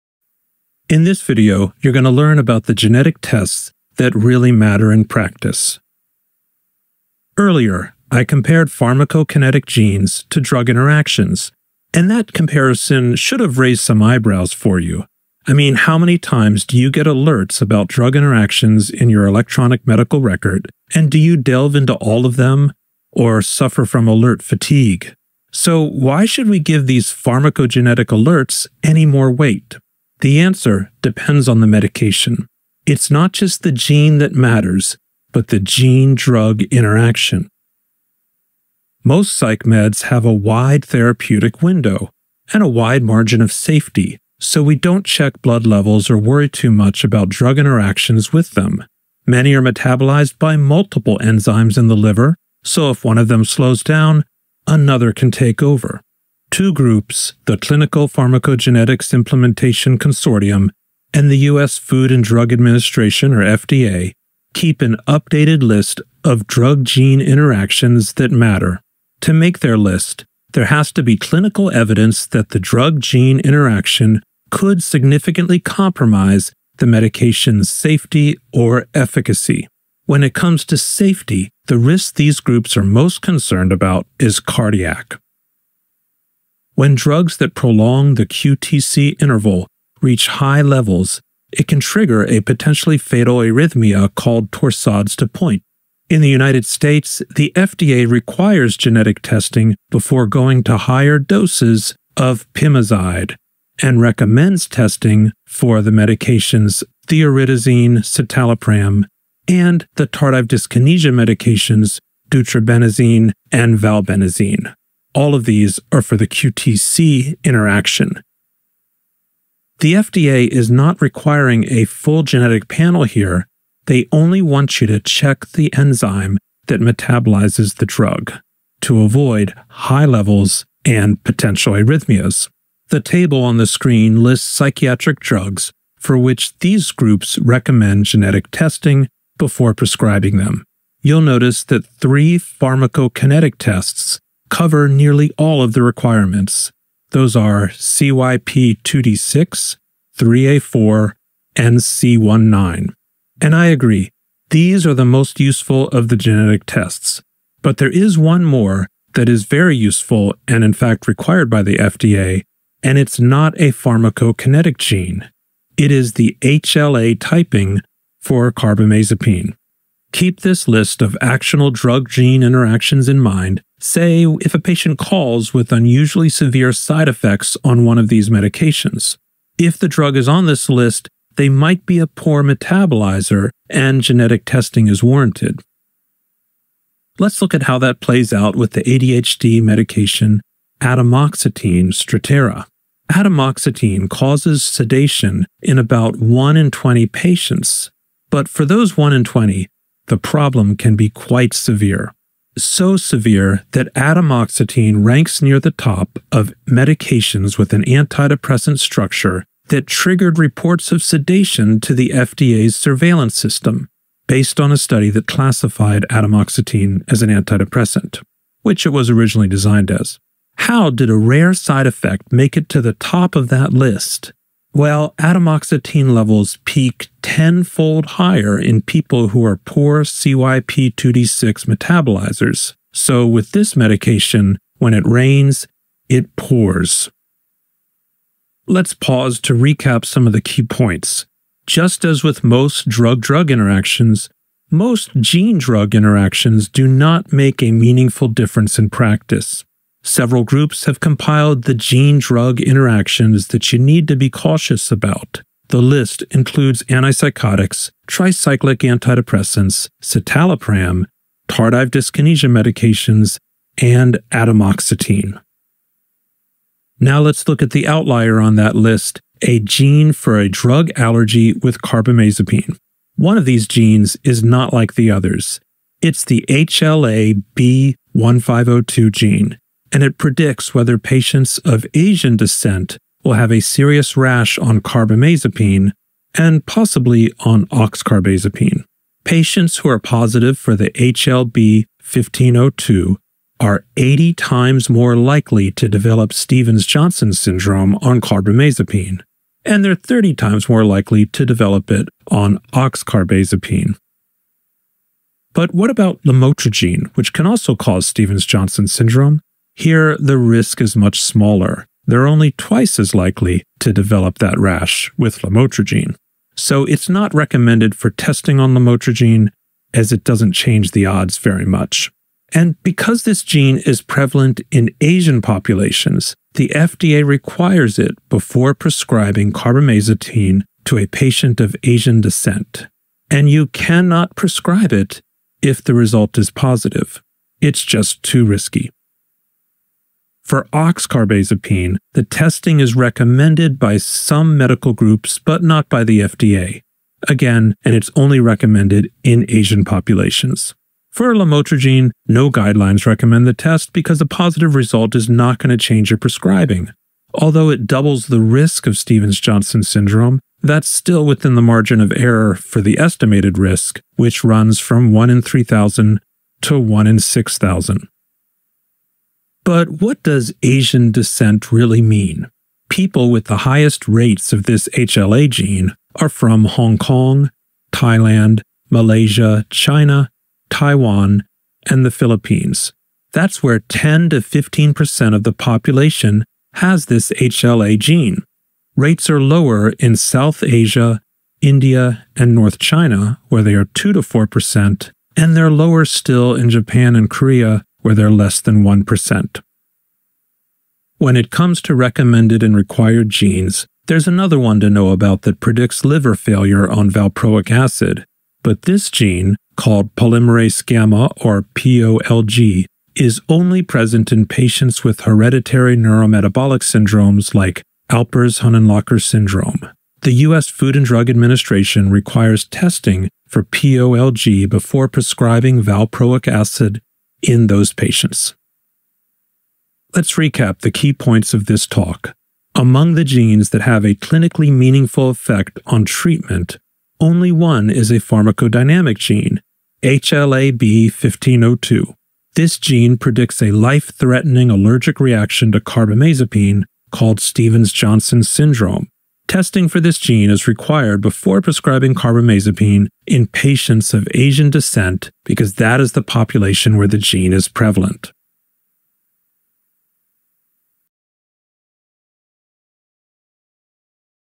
Pharmacogenetic Tests That Matter in Psychiatric Practice
This is an excerpt from a video lecture.

If you become a member, you can earn CME credits for this activity. Want to know more? Take a look at the Psychopharmacology Update